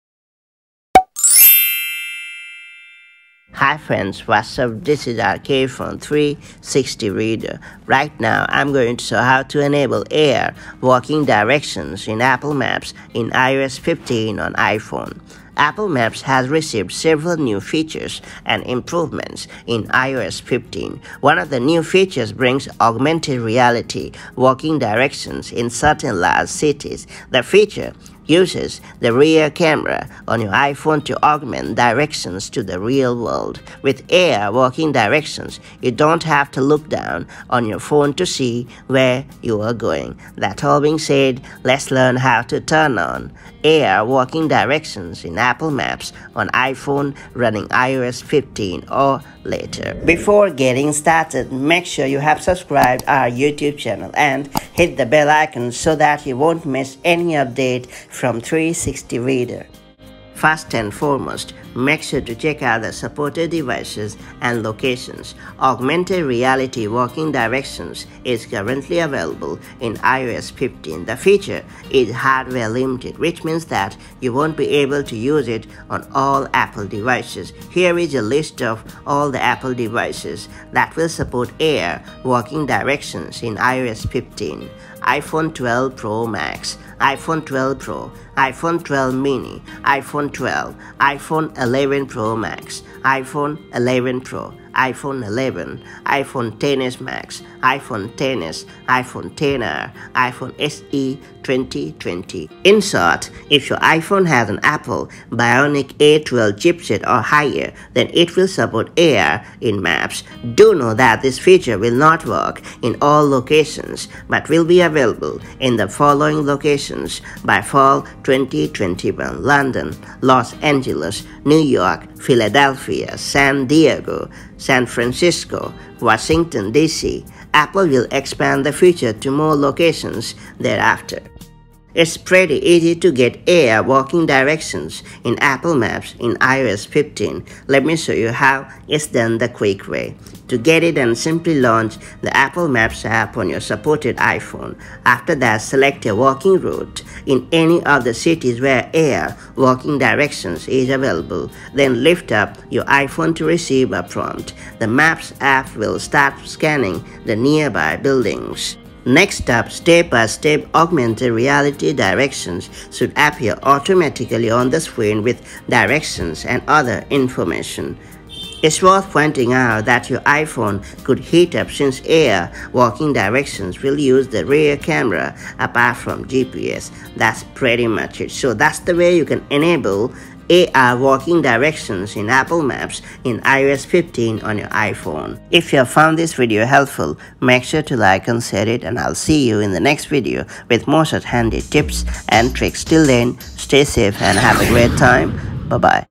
How to Enable AR Walking Directions in Apple Maps in iOS 16 on iPhone
Here is how you can enable AR walking directions in Apple Maps in iOS 16 on iPhone. Let’s find out how this all-new iOS 16 feature works!